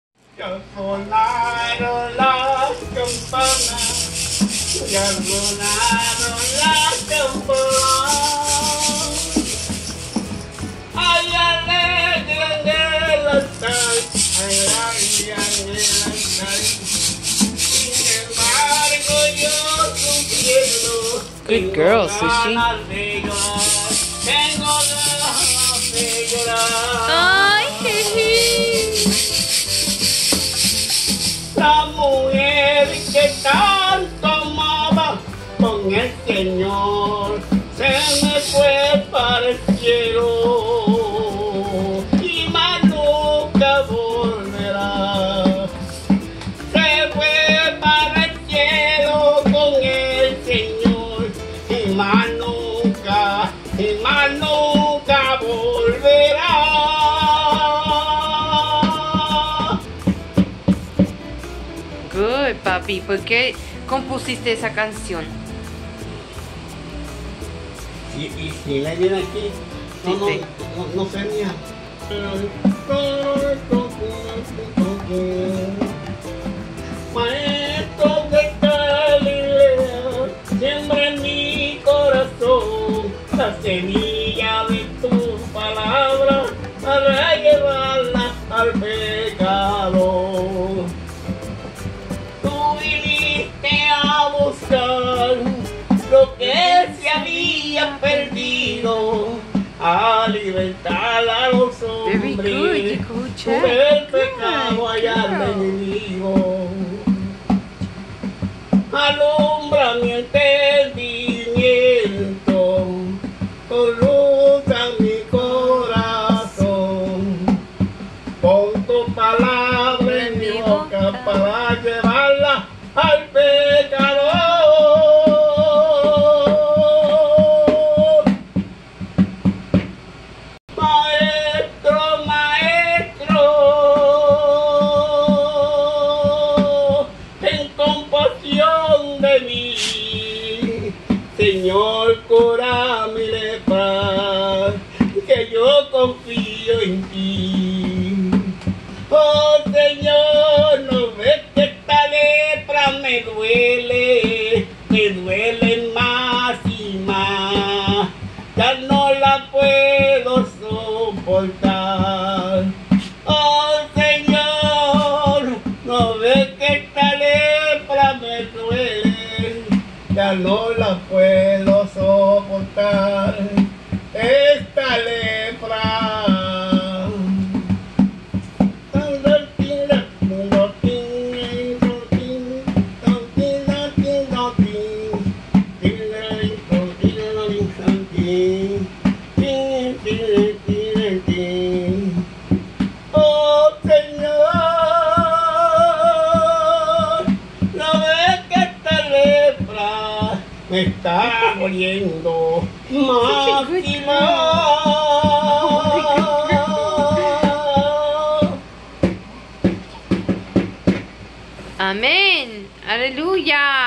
Pops & His Homemade Acoustic Sound Machine
Just an old video from somewhere around Feb 2023 or so, I forgot to upload to collection.